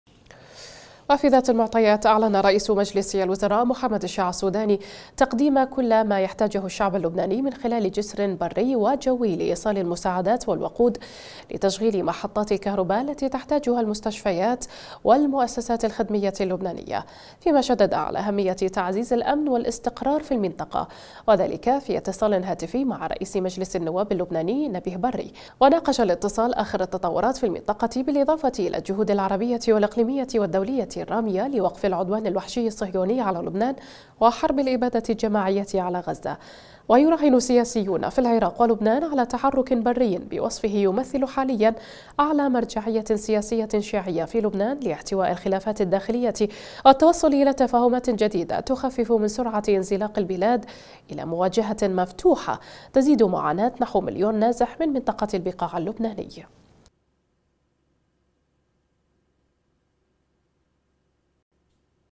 السوداني يهاتف بري لتأكيد الدعم العراقي للبنان
وفي ذات المعطيات أعلن رئيس مجلس الوزراء محمد شياع السوداني تقديم كل ما يحتاجه الشعب اللبناني من خلال جسر بري و جوي لايصال المساعدات والوقود لتشغيل محطّات الكهرباء التي تحتاجها المستشفيات والمؤسسات الخدمية اللبنانية، فيما شدد على أهمية تعزيز الأمن والاستقرار في المنطقة، وذلك في اتصال هاتفي مع رئيس مجلس النواب اللبناني نبيه برّي، وناقش الاتصال آخر التطورات في المنطقة بالإضافة إلى الجهود العربية والإقليمية والدولية الرامية لوقف العدوان الوحشي الصهيوني على لبنان وحرب الإبادة الجماعية على غزة، ويراهن سياسيون في العراق و لبنان على تحرك بري بوصفه يمثل حاليا أعلى مرجعية سياسية شيعية في لبنان لاحتواء الخلافات الداخلية و التوصل الى تفاهمات جديدة تخفف سرعة انزلاق البلاد الى مواجهة مفتوحة تزيد معاناة نحو مليون نازح من منطقة البقاع اللبناني.